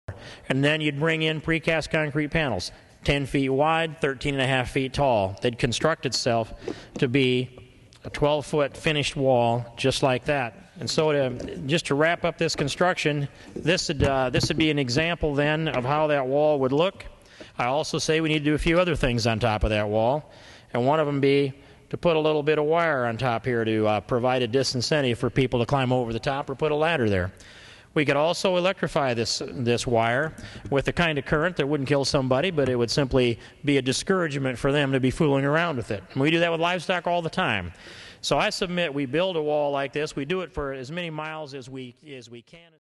The King of the Electric Border Wall
Congressman King wants to electrify the U.S. Mexico border wall